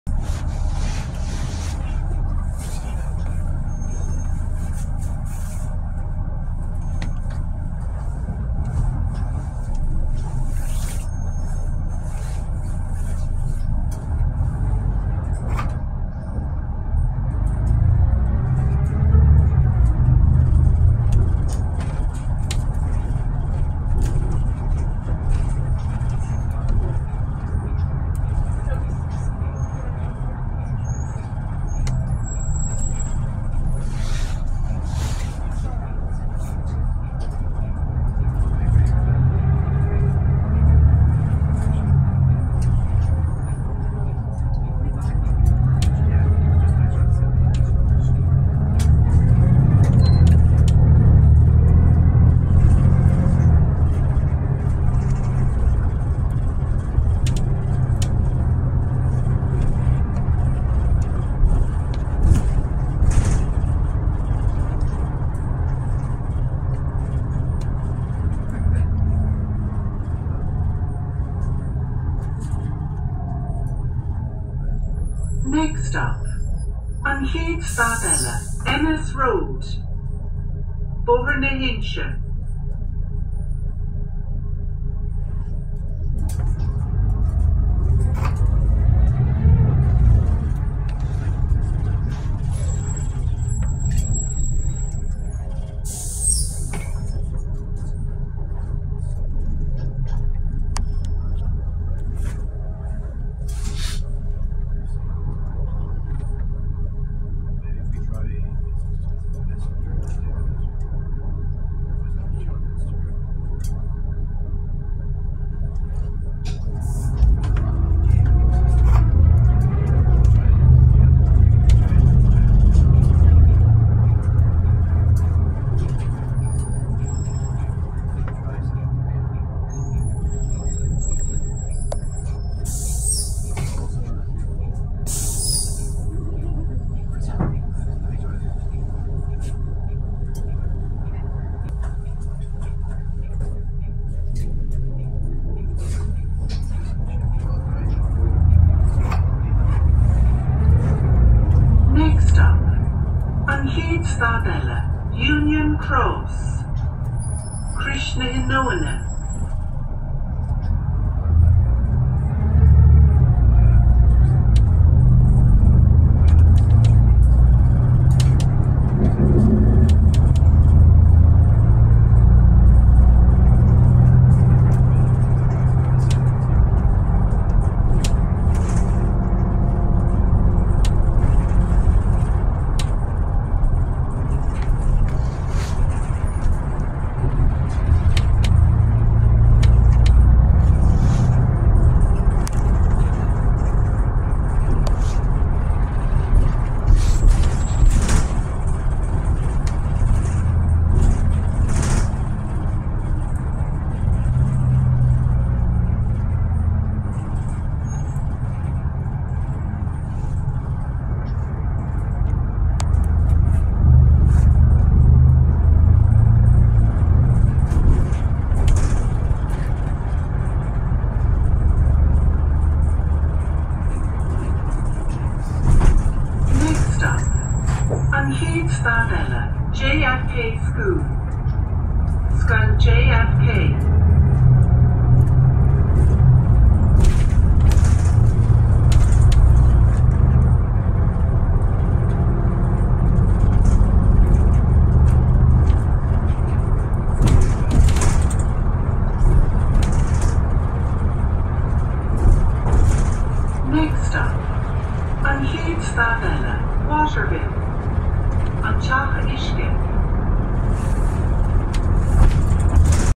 View from the Only Deck: VB485 on Route 317 from Henry Street, Limerick out the Ennis Road.
A 4:23 recording of NTA/Bus Éireann (Roxboro Depot, Limerick) Volvo B8RLE Sunsundegui SB3 VB485 (212-D-24706) as captured on Route 317 from Henry Street, Limerick out the Ennis Road on Saturday morning, 19th October 2024.

Routes 317and 317A were introduced in mid-August 2024, linking Cratloe, Limerick and Ennis via Sixmilebridge and Quin parallel with high-ish frequency Routes 330 and 343.

As is all too often the case, my phone stopped recording of its own volition but at least the recording was saved this time. I had planned a 5-minute recording.